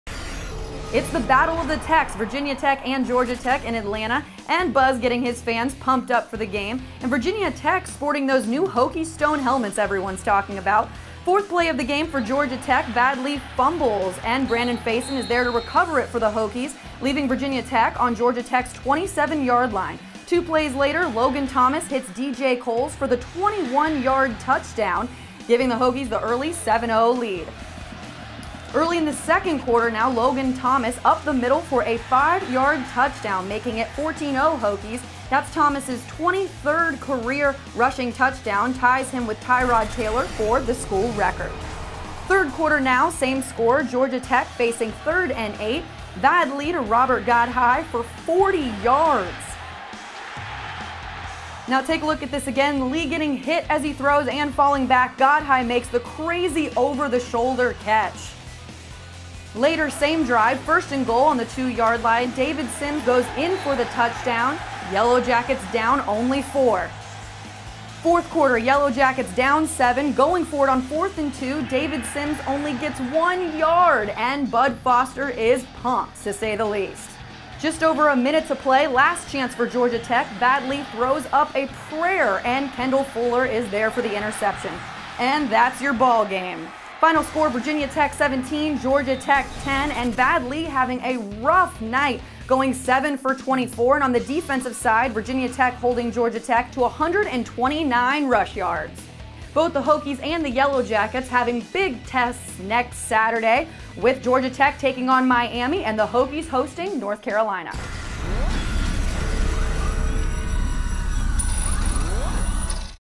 2013 ACC Football Highlights | Virginia Tech vs Georgia Tech | ACCDigitalNetwork
Virginia Tech moved to 4-1 on the season Thursday night with a 17-10 road win against Coastal Division rival Georgia Tech.  The Hokies were led by Logan Thomas who went 19 of 25 for 221 yards and a touchdown.  Thomas also ran for 58 yards and an additional touchdown.  The Yellow Jackets had chances to even the score in the 4th quarter, but a stop on 4th and 2 helped the Hokies seal the win.

2013 ACC Football Highlights | Virginia Tech vs Georgia Tech | ACCDigitalNetwork